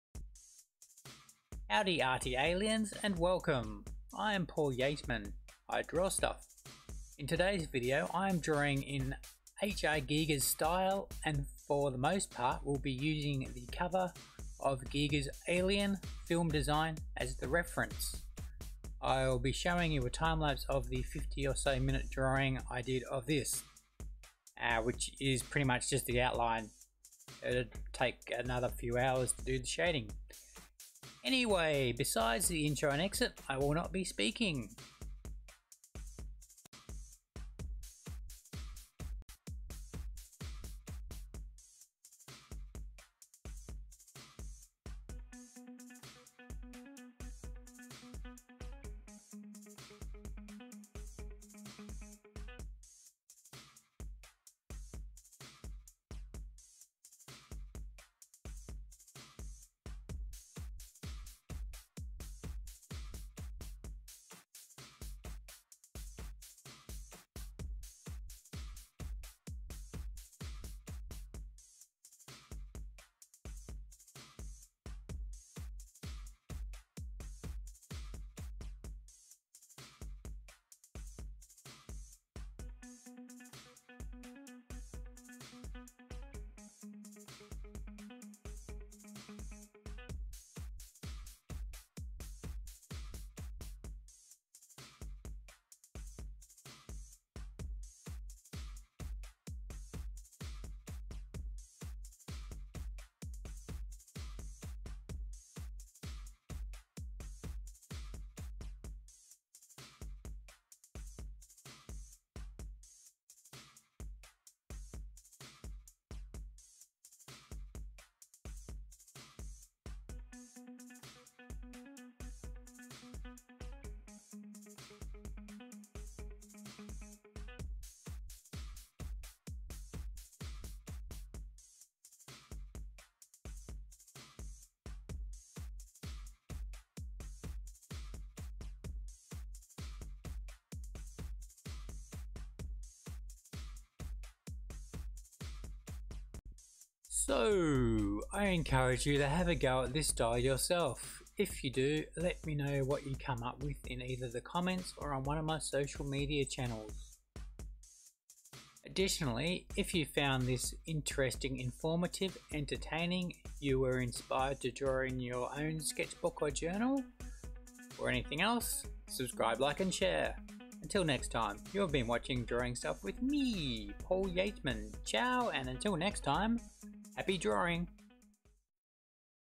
Draw HR Giger’s Alien | Biomechanical Style
In today’s video I am drawing in a HR Giger’s style and will be for the most part, using the cover or Giger’s Alien – Film Design as the reference.  I will be showing you a time-lapse of a 50 or so minute drawing of this piece.
Besides the intro and exit, I will not be speaking.

CHAPTERS
0:00 Intro Screen
0:20 Sketching
2:38 End Screen

Get social with me...